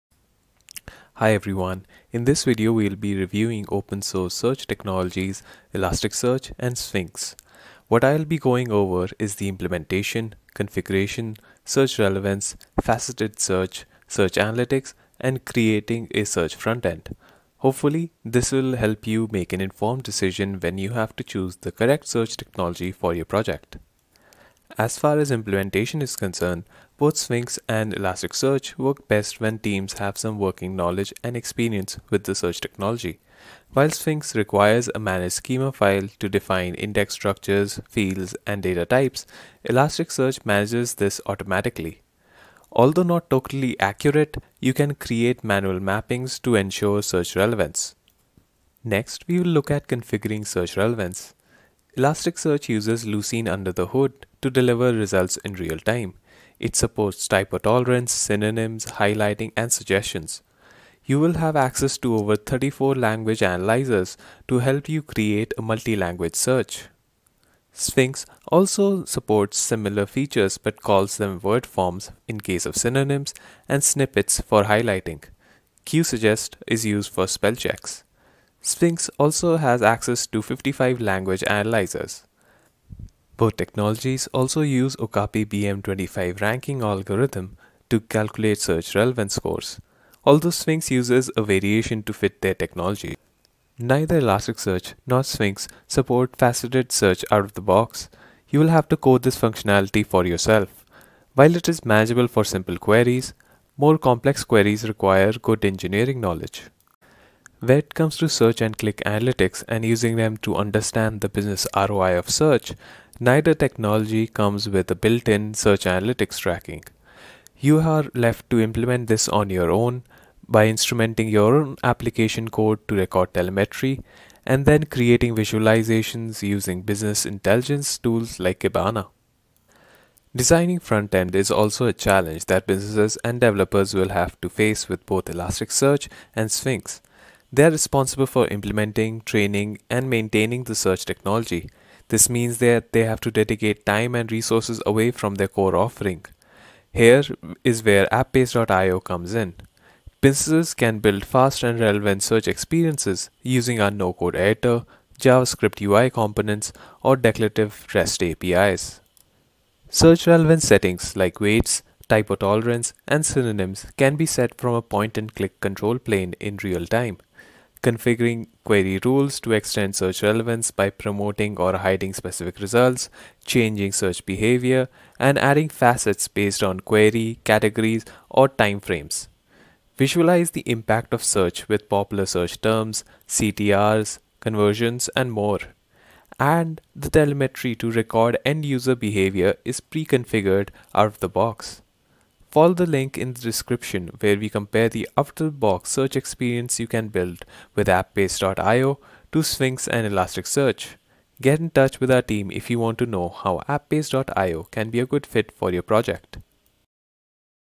Comparing Elasticsearch and Sphinx
In this video we compare the capabilities of Elasticsearch and Sphinx (SQL Phrase Index), and discuss possible challenges a business can face when designing their search with these technologies.